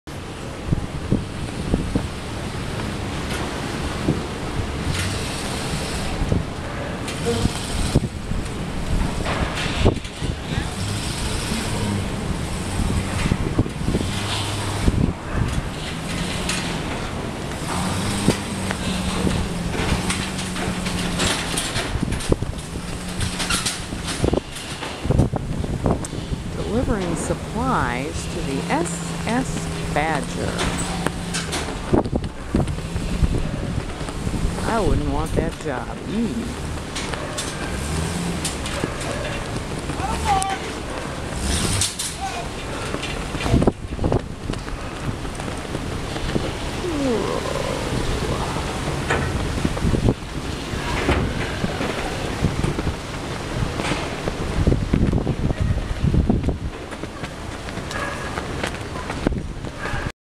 supplies for the S S Badger
Supplies delivered to the S.S. Badger. This spiderwork of timbers looks too flimsey to take the weight of a truck. Those who bring in the goods are well practised backer uppers!